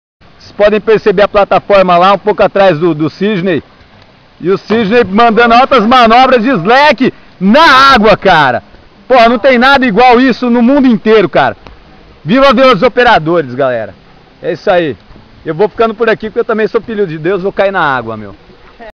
Slack line na água Vila dos Operadores_0002
Mais um pouco do grande amigo Sydney Forster detonando no slackline no Rio Paraná